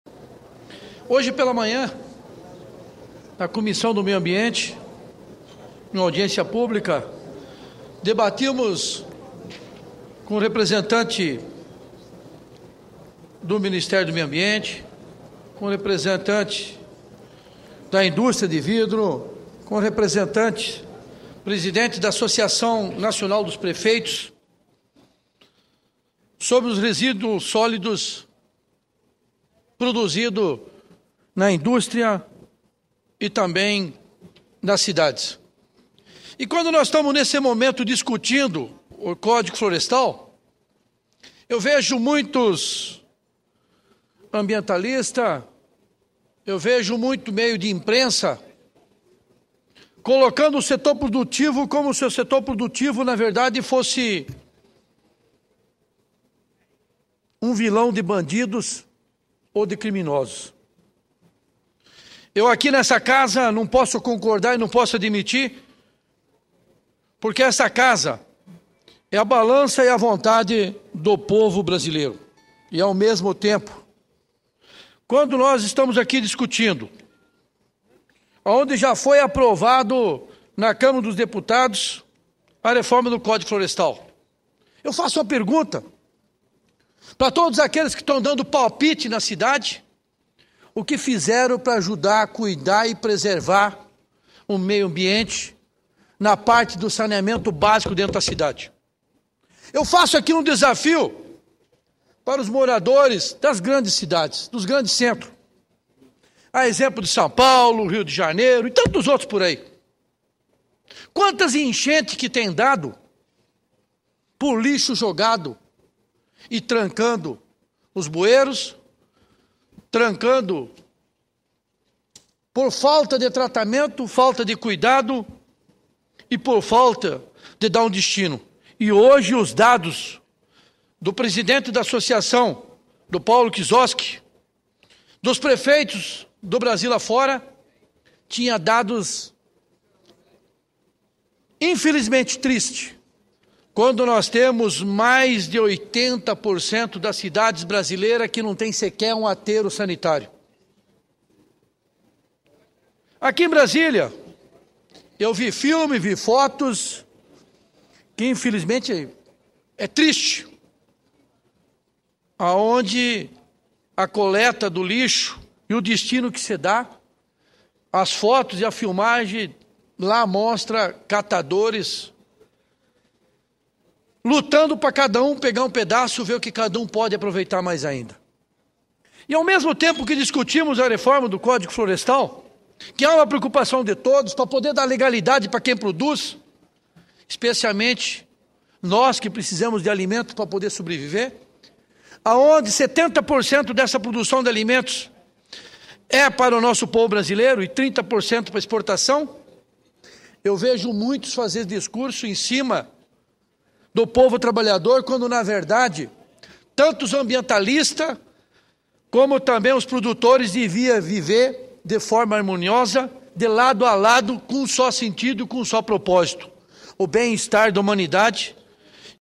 A falta de saneamento básico nas cidades também prejudica o meio ambiente, alerta sen. Ivo Cassol
28/06/2011 - Plenário - A falta de saneamento básico nas cidades também prejudica o meio ambiente, alerta sen. Ivo Cassol